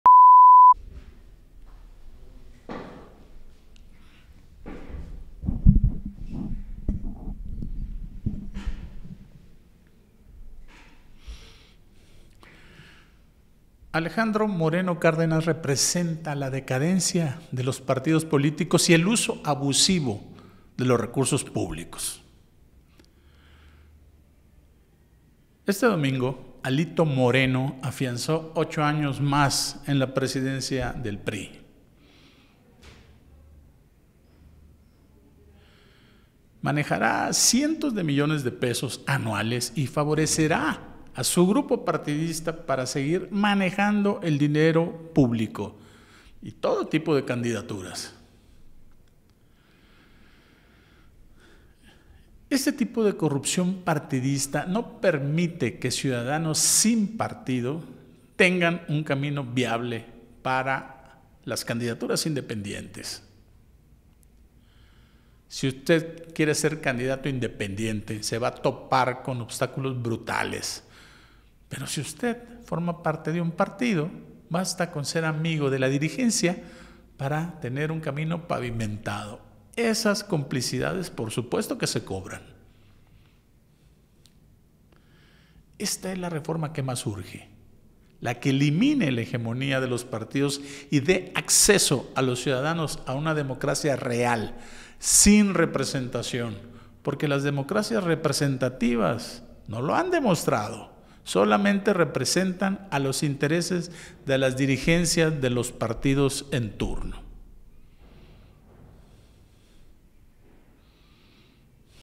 Rosa Icela (SSPC) recibirá a Collins para revisar la verdad del caso
Rosa Icela Rodríguez (SSPC) recibirá a Raymundo Collins para revisar la verdad del caso: AMLO